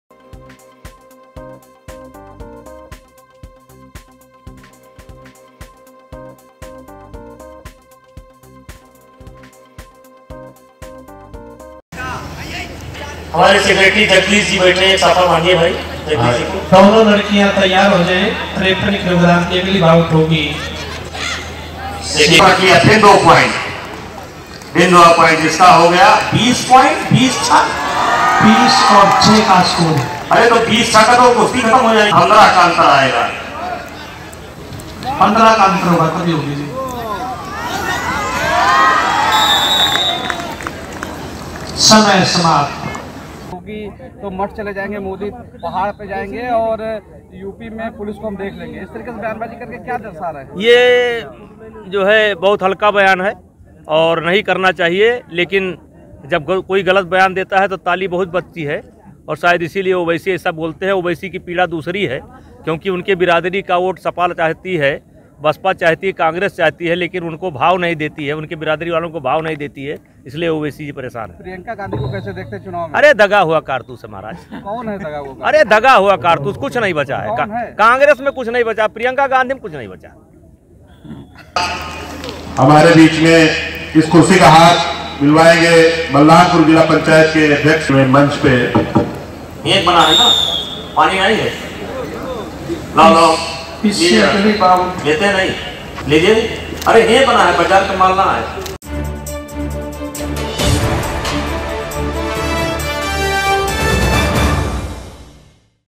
ओवैसी पर बोले सांसद बृजभूषण, कांग्रेस को कहा दगा हुआ कारतूस, जानें क्यों
गोंडा कांग्रेसमें अब कुछ बचा ही नहीं है। प्रियंका गांधी वाड्रा सहित पूरी कांग्रेस पार्टी दगा हुआ कारतूस है। यह बात कैसरगंज से सांसद व कुश्ती संघ के राष्ट्रीय अध्यक्ष बृजभूषण शरण सिंह ने सीनियर कुश्ती चैंपियनशिप आयोजन के मौके पर पत्रकारों से रूबरू होते हुए कहा। उन्होंने ओवेसी द्वारा यह कहे जाने पर प्रधानमंत्री मोदी पहाड़ पर मुख्यमंत्री योगी मठ में चले जाएंगे। इस पर पलटवार करते हुए कहा बहुत हल्का बयान है।